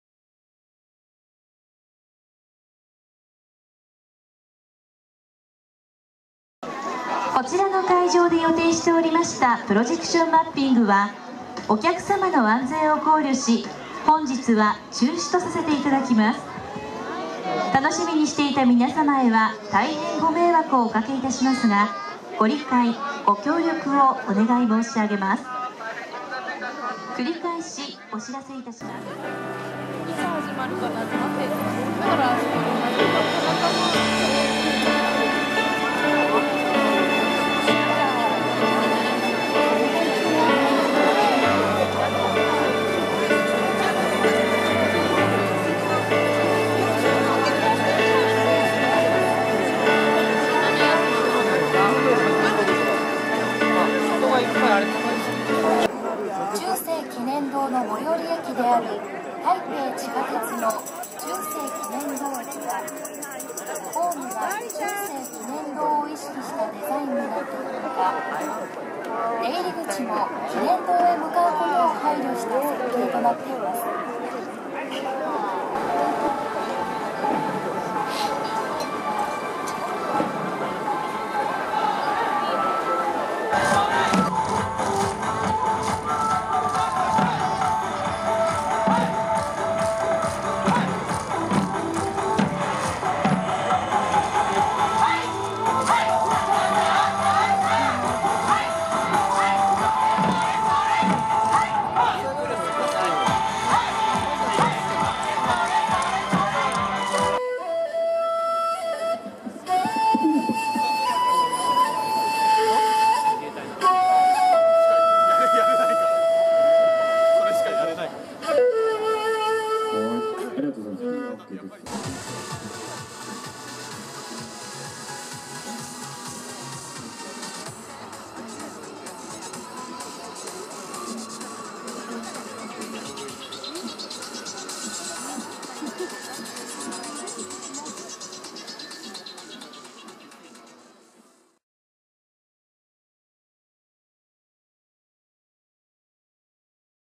さっぽろ雪まつり 夜の大通会場 大混雑で中止のイベントも！！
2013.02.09　三連休の初日、日中の雪も風も止んで
大通会場はものすごい人出で賑わっていました。

５丁目会場で話題となっているプロジェクションマッピングは
あまりの人気で　安全確保ができないという理由で中止になってしまいました。
映像が投影されるはずだった会場では　よさこいソーランが演じられました。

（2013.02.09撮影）